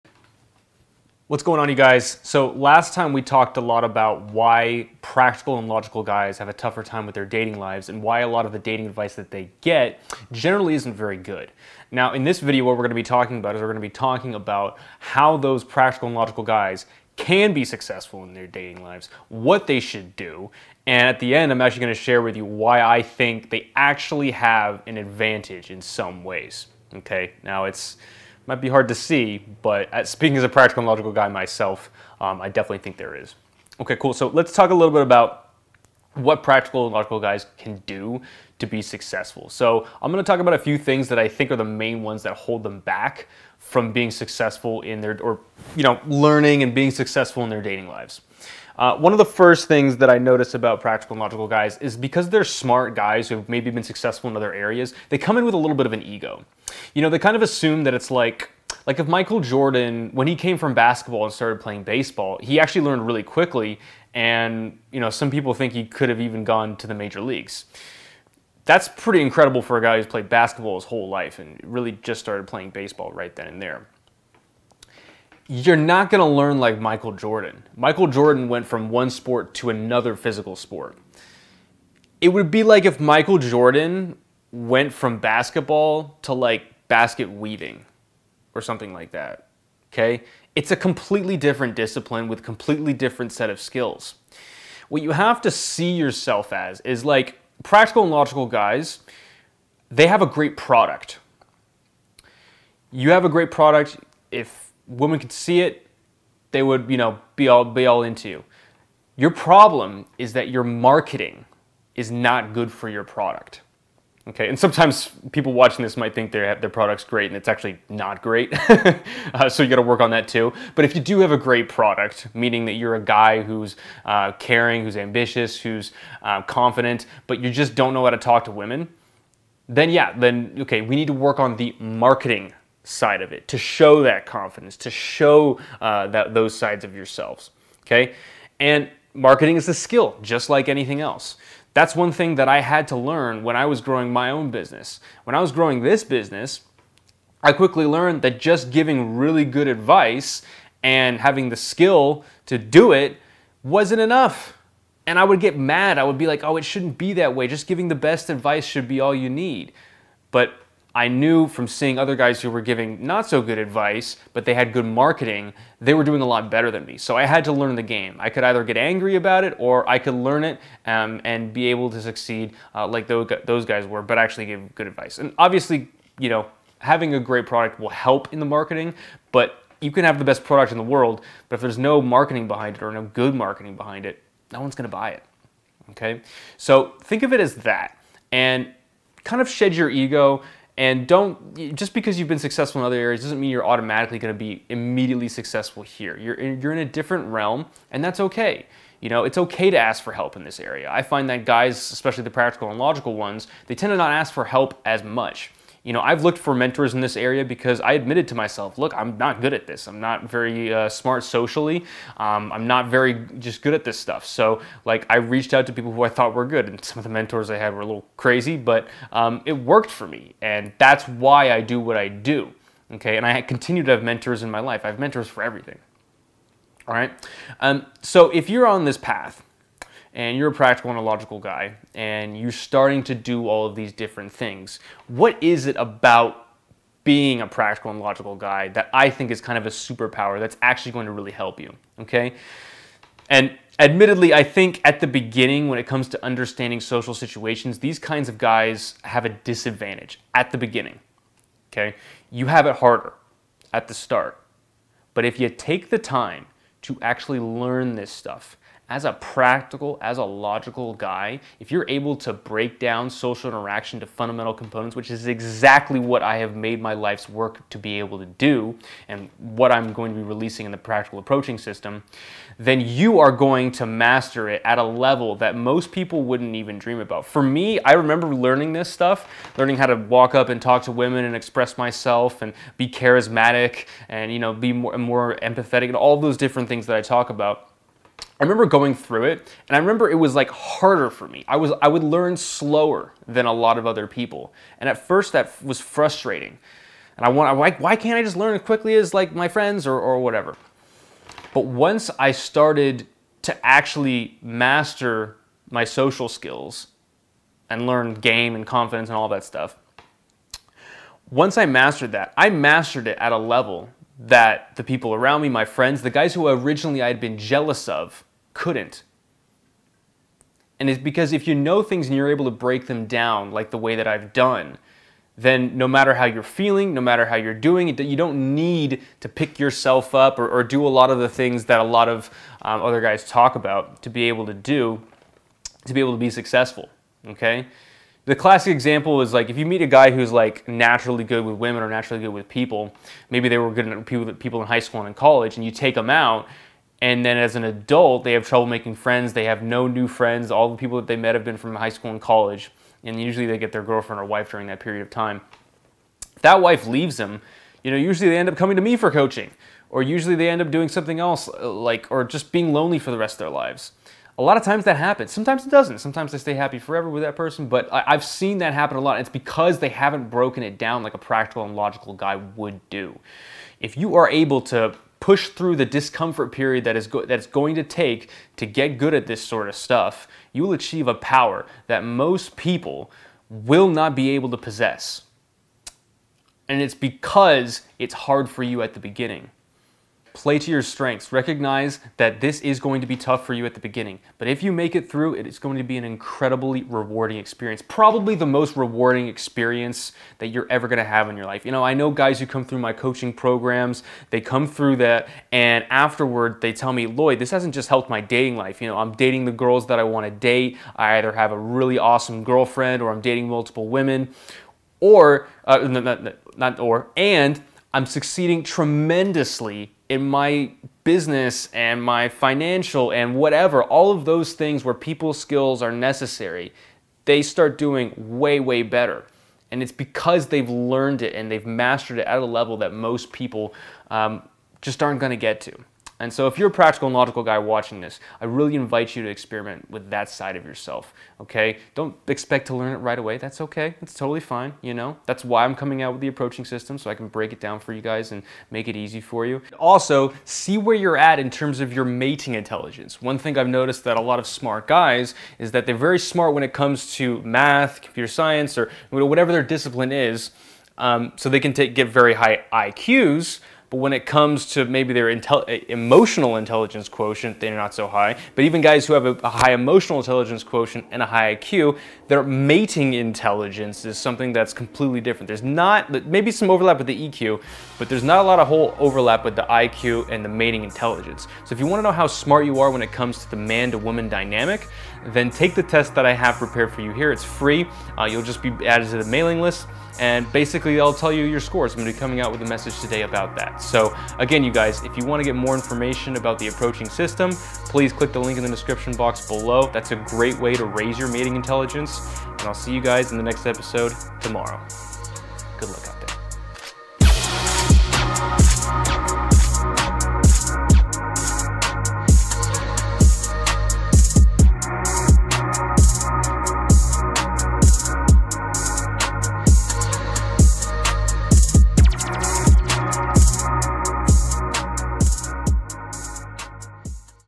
How logical and practical guys can succeed with women
Logical guys have it tough when it comes to dating... at first. I explain how to succeed if you consider yourself a logical and practical guy.

  / a.single.guy